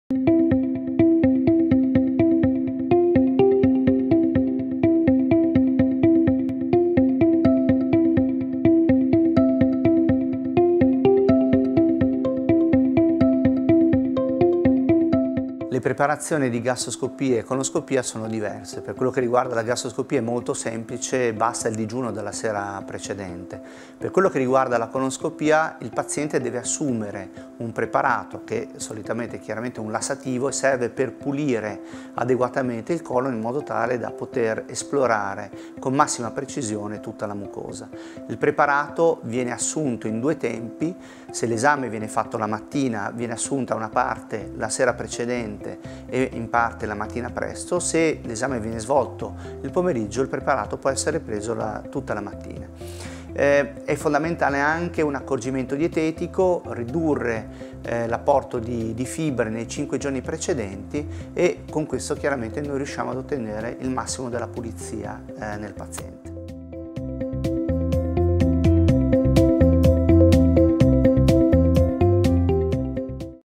Endoscopia digestiva: come prepararsi all'esame | Top Doctors
Devi fare una gastroscopia o una colonscopia? Il Prof. Luigi Ricciardiello, esperto in Gastroenterologia, ci spiega quale preparazione necessita un'endoscopia digestiva.

Vorresti avere più informazioni?